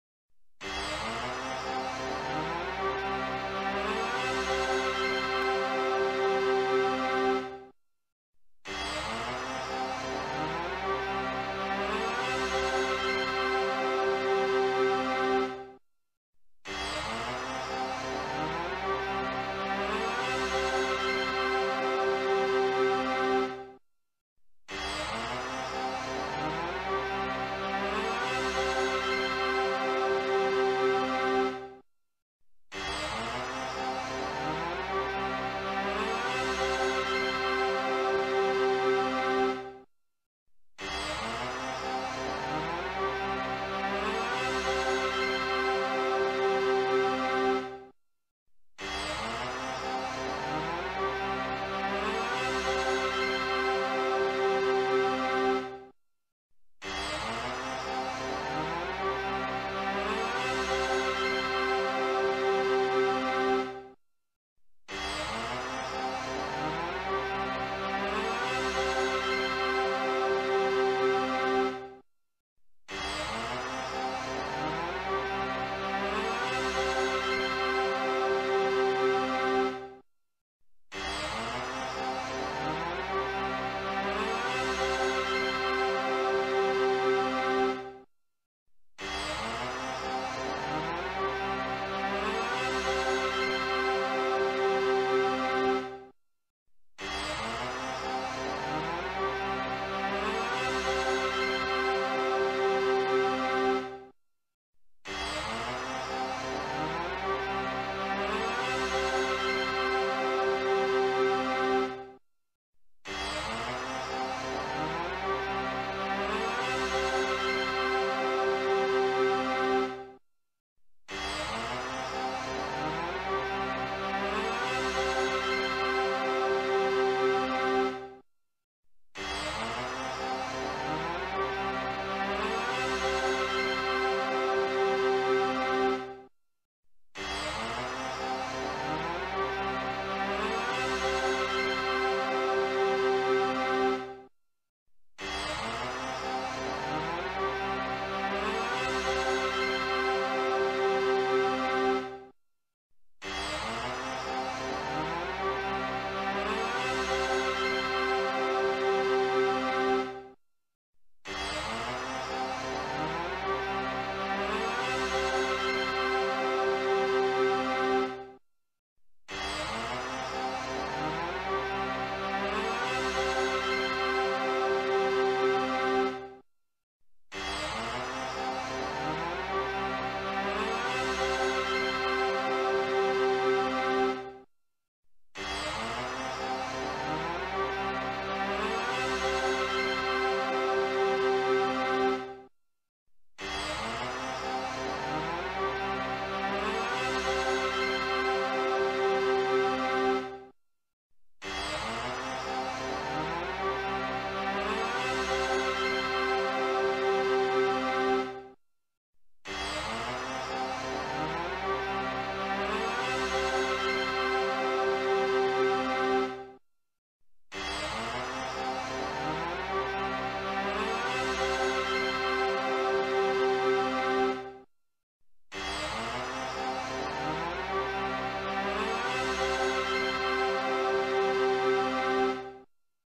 [#175] Messing Around With Logos - Episode 107: SEGA Saturn (Japan)
Effects on the SEGA Saturn startup from Japan.